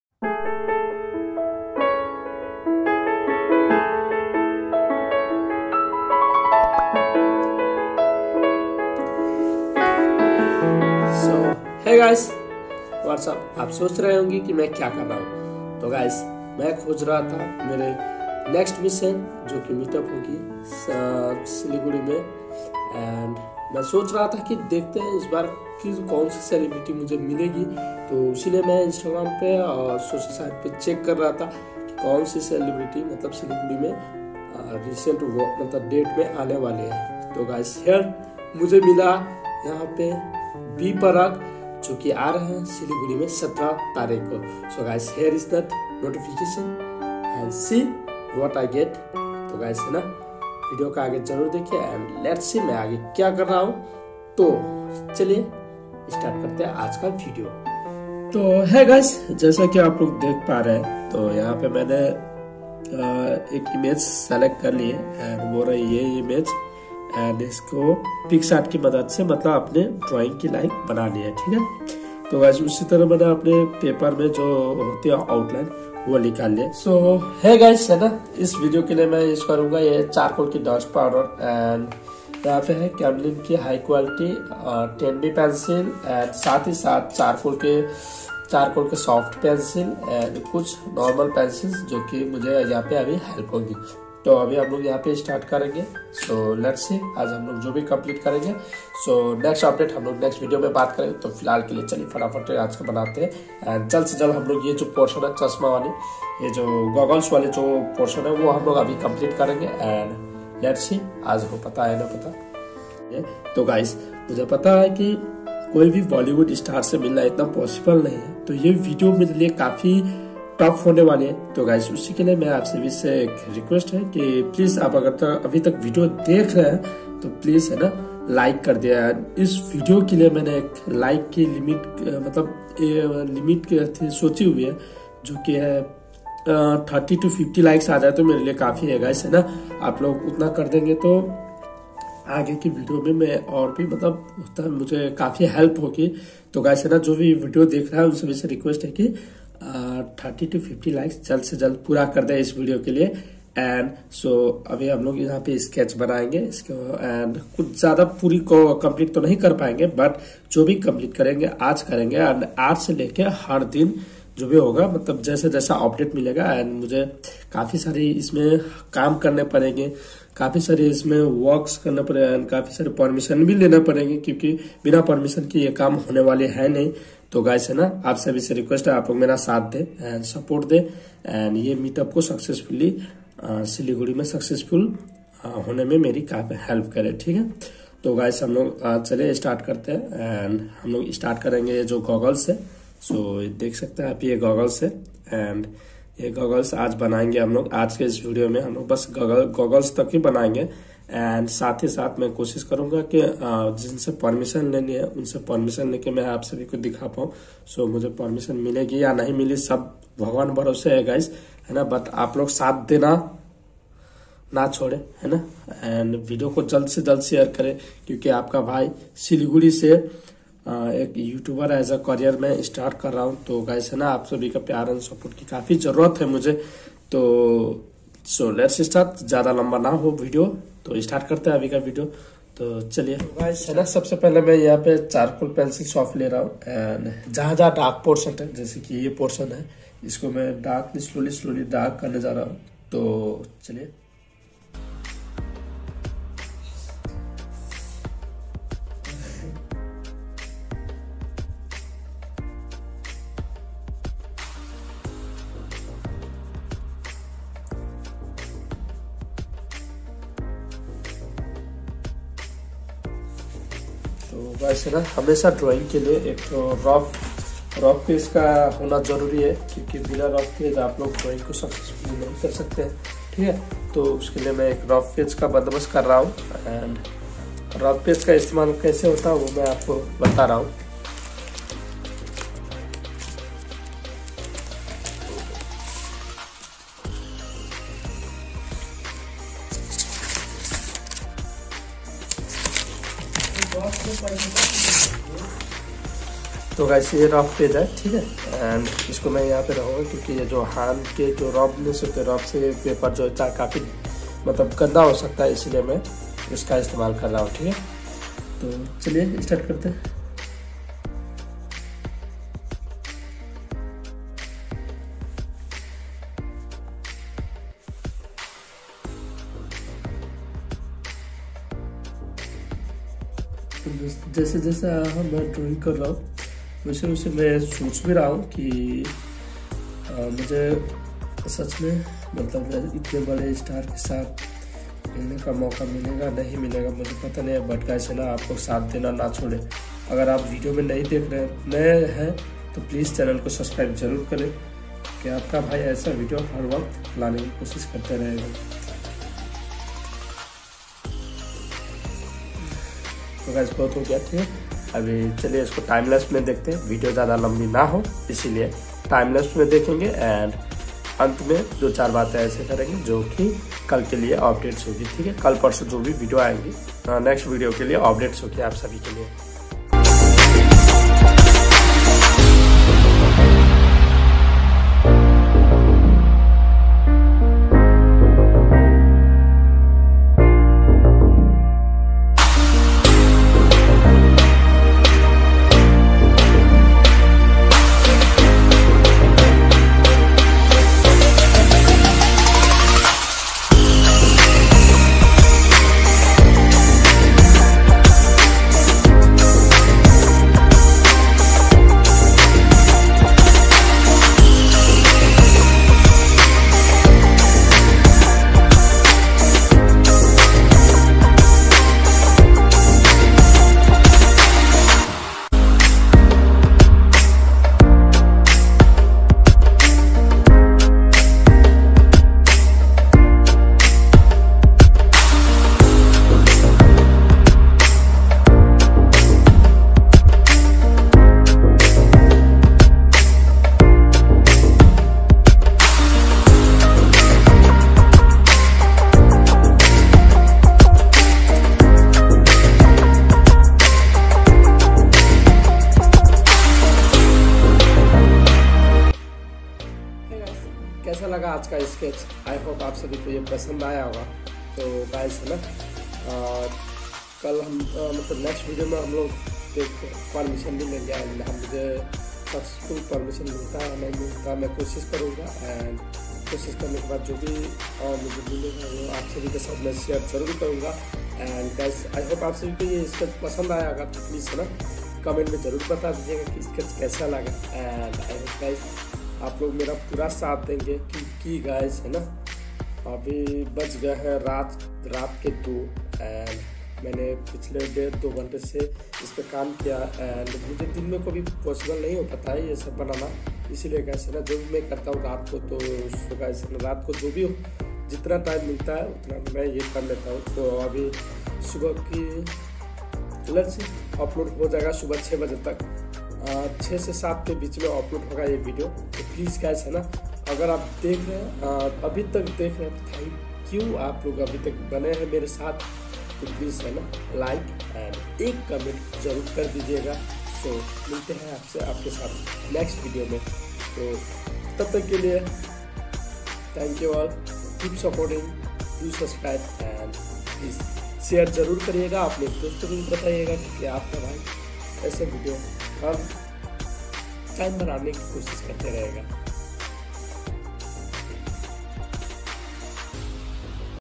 B-PARAK LIVE | Meet-Up challenge for Mitlesh Art 2.0 | mission to meet B-PARAK
Kya SILIGURI me BOLLYWOOD Star se meet up hogi success on 17th December B-PARAK..